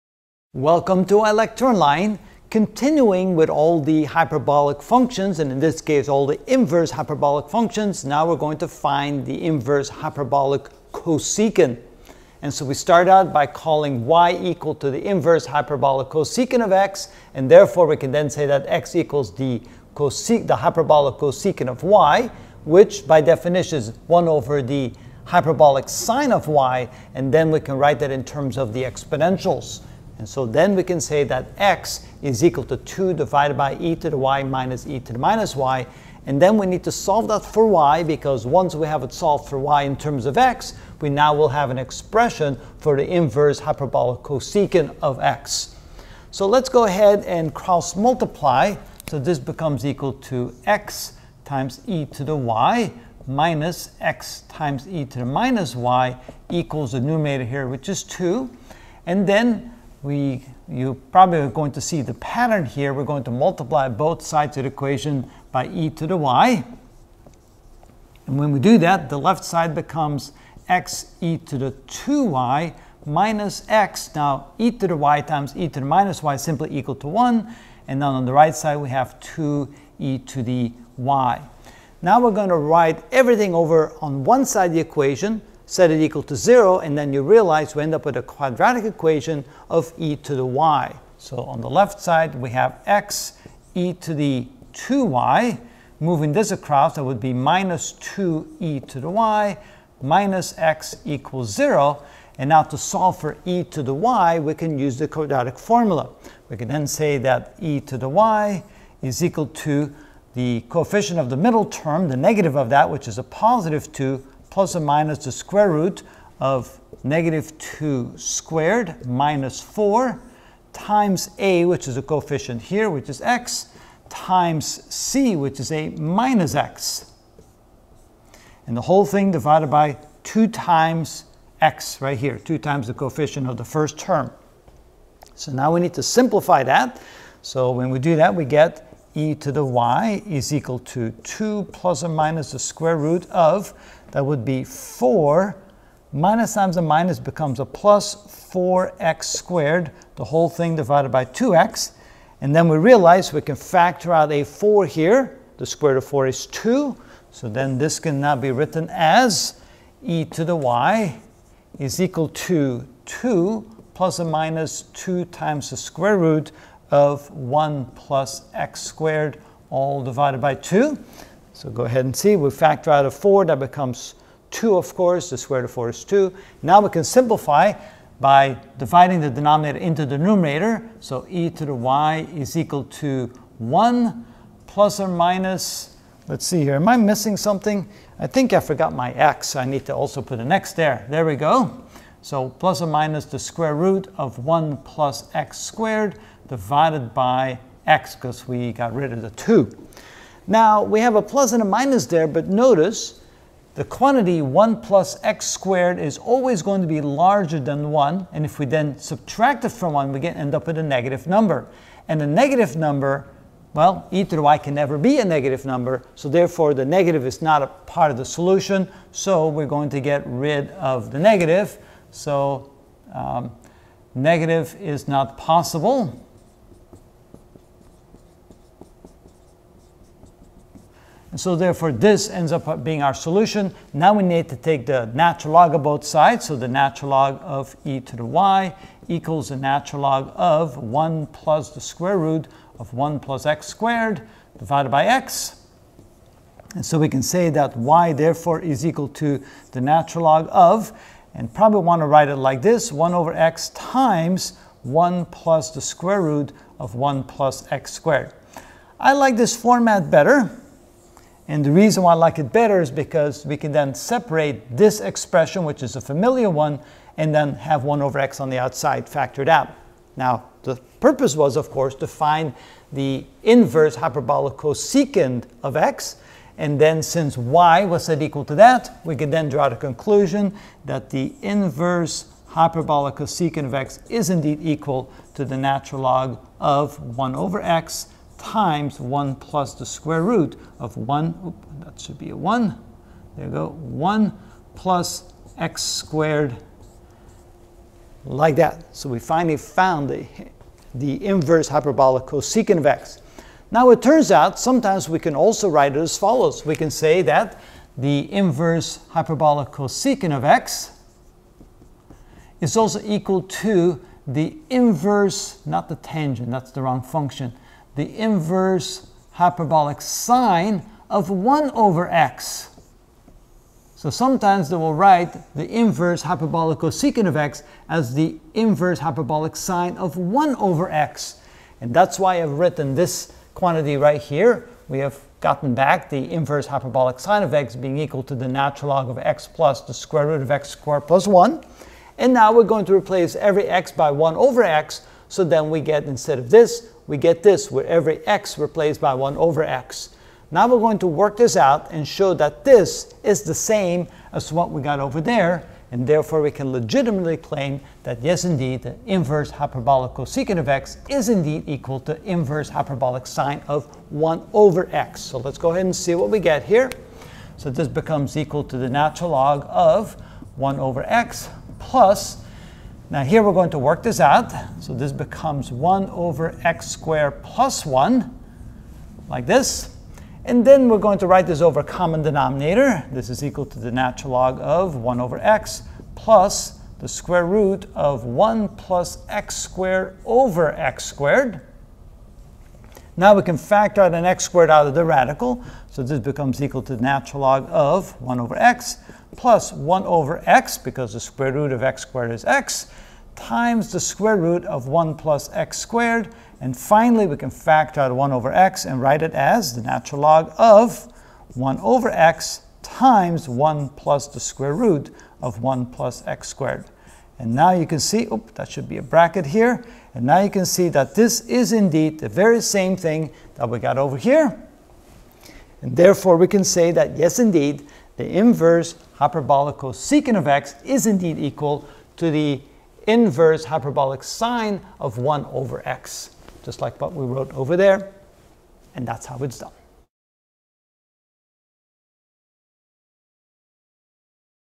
Calculus 2: Hyperbolic Functions (38 of 57) Finding arc(cschx)=?
In this video I will find y=(csch)^-1(x)=? or y=arccsch(x)=?